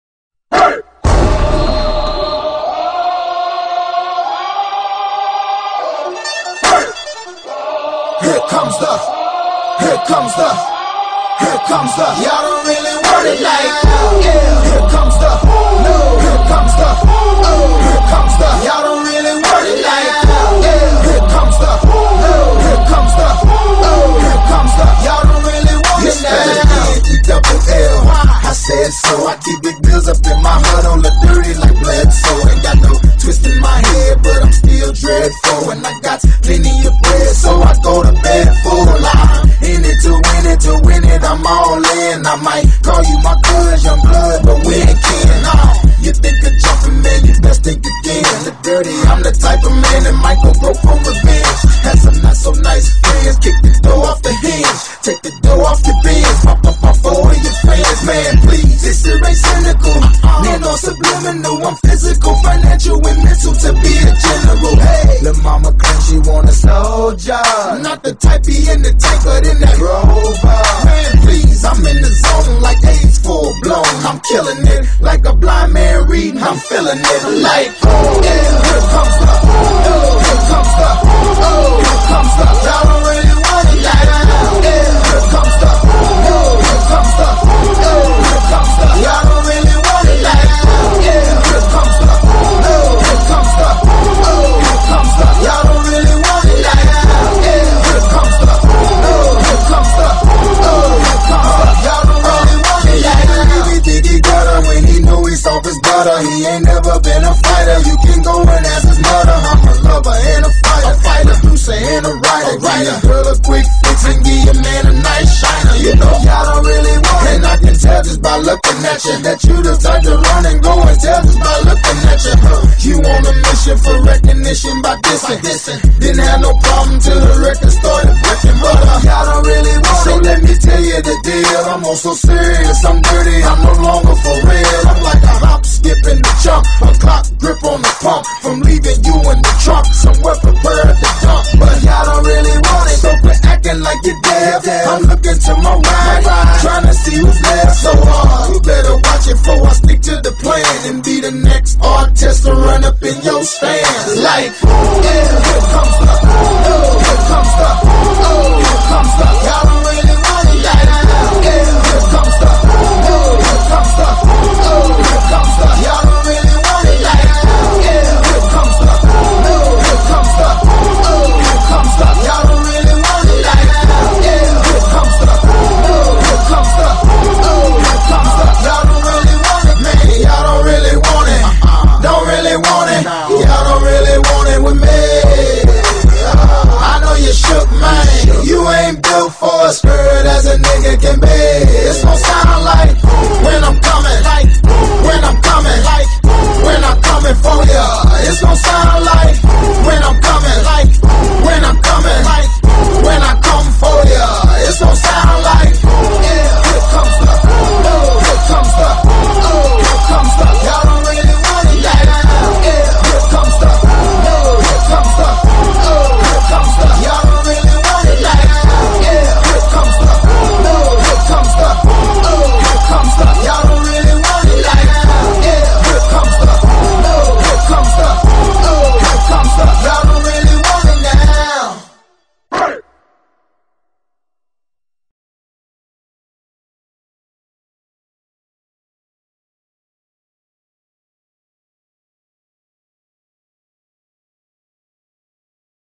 Stephen Hill Highlights
Highlights from Stephen Hill in college and his combine workout. He was the 43rd draft pick of the NFL draft in 2012 by the New York Jets. He also had the fastest 40 yard dash.